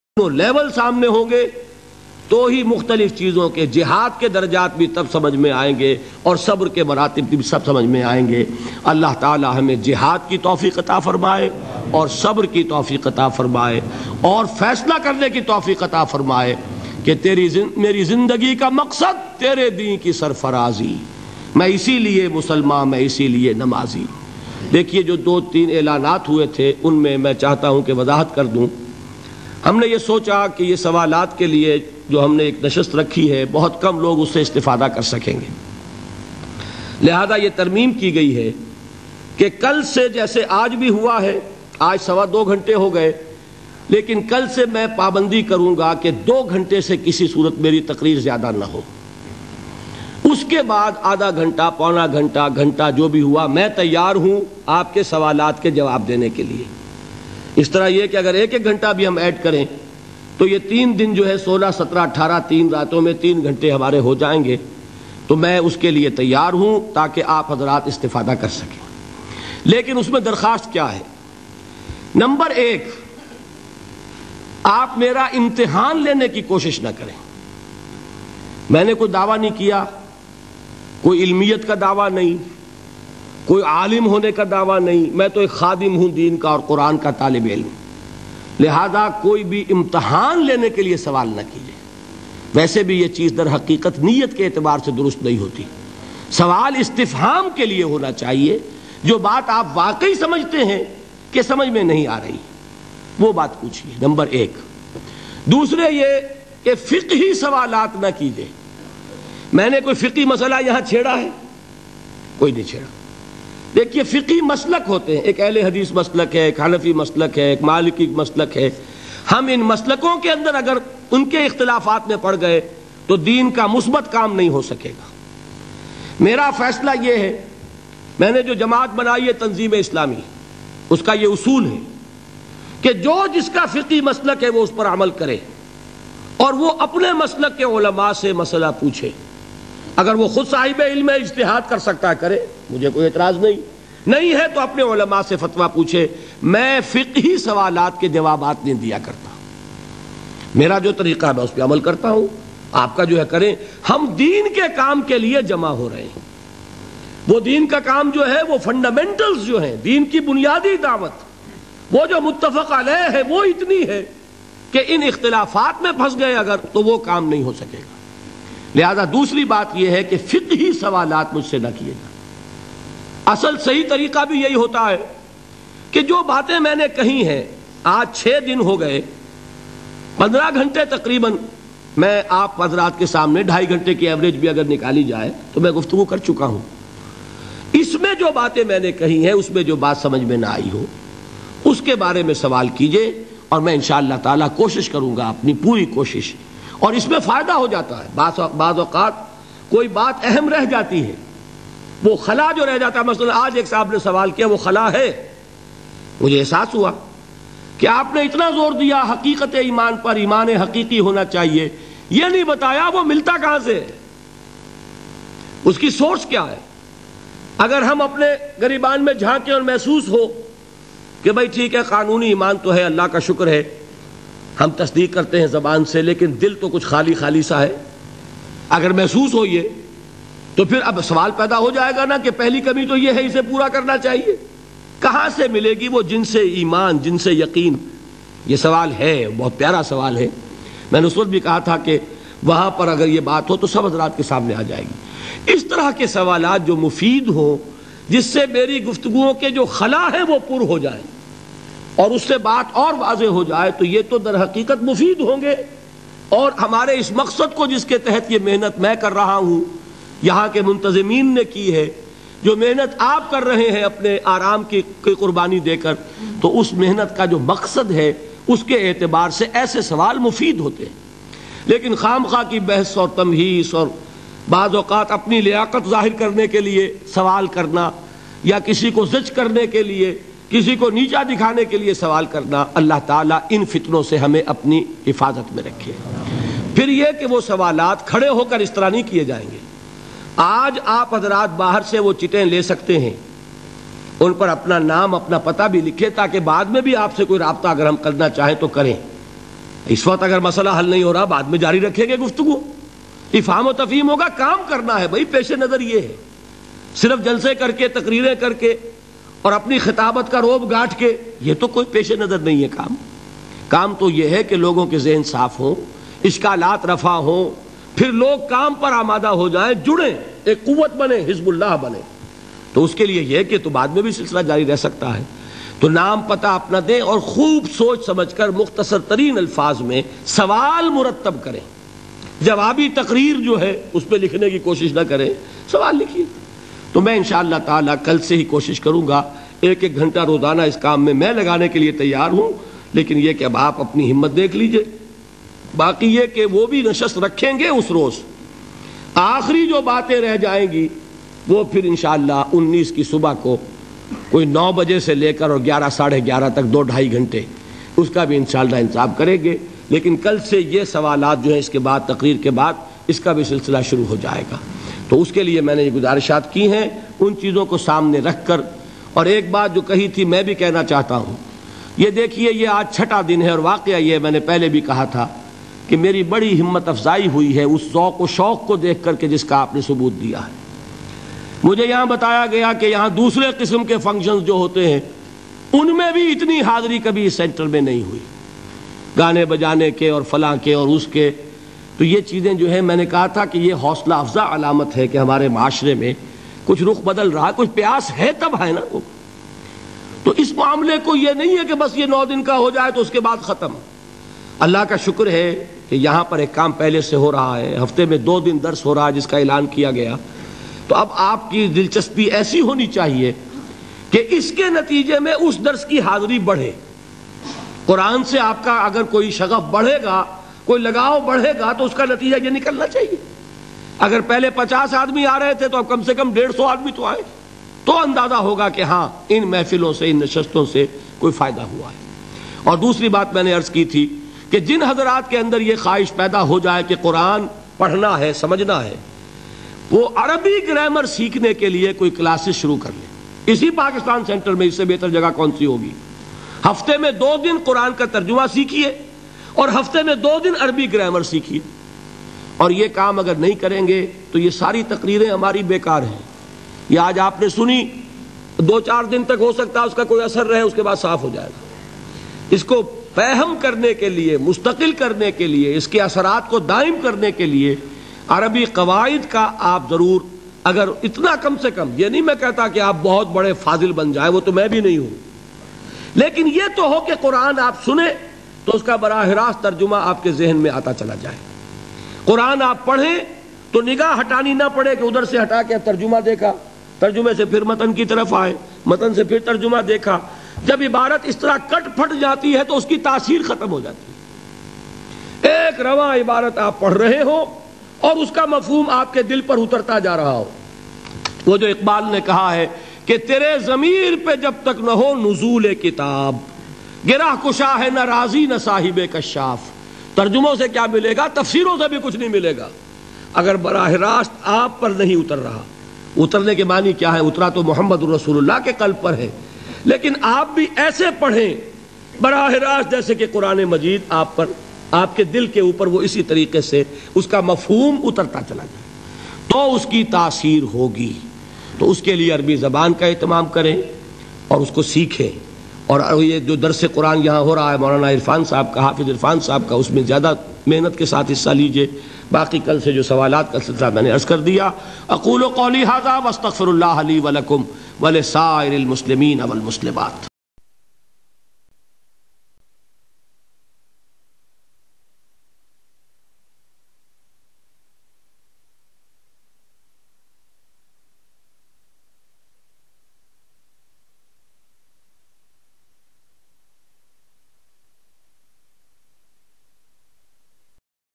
(16/16) Tawasi Bis Sabar (Abu Dhabi) Dr. Israr Ahmed
A Public Lecture by Dr. Israr Ahmed at Abu Zahbi U.A.E

Tanzeem-e-Islami Quran Academy Markazi Anjuman e Khuddam ul Qur'an Lahore IRF Peace TV Urdu Qtv Dr. Zakir Naik